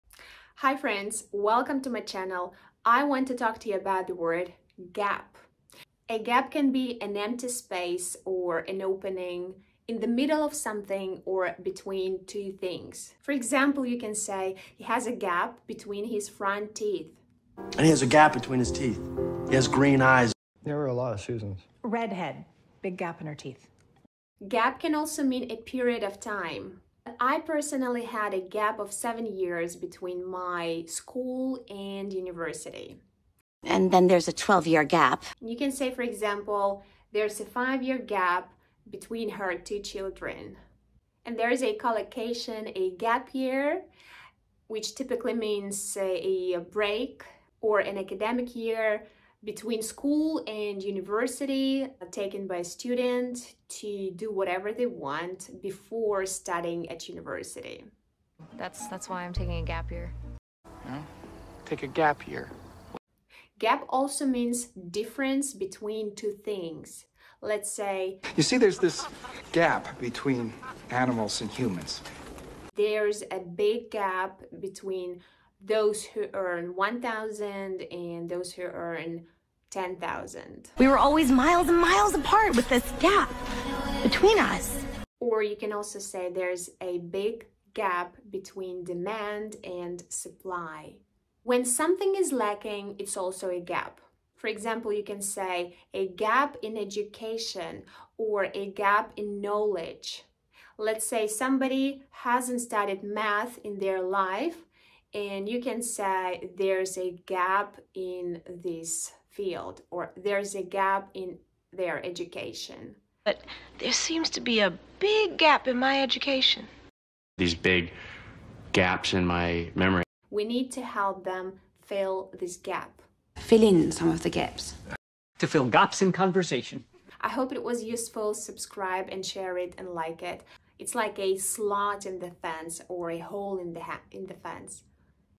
GAP. Meaning of the word GAP!
A gap can mean: 1. An empty space. 2. A period of time (doing something different or when something doesn’t happen) 3. A difference 4. When something is missing or lacking.